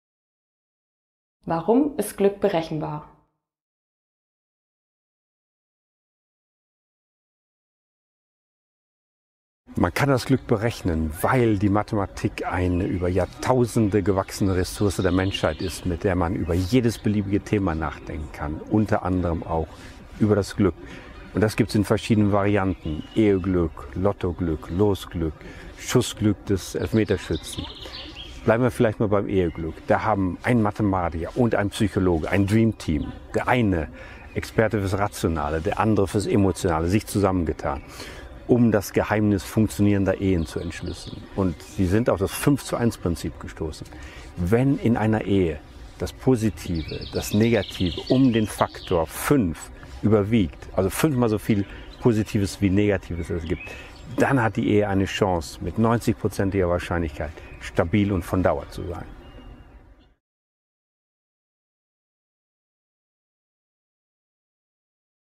1. Stuttgarter Wissenschaftsfestival: Warum ist Glück berechenbar?
Wissenschaft anschaulich erklärt: Prof. Dr. Christian Hesse erläutert am Beispiel funktionierender Partnerschaften, warum Glück berechenbar ist. Mehr davon gibt's beim 1. Stuttgarter Wissenschaftsfestival vom 26. Juni bis 6. Juli 2019.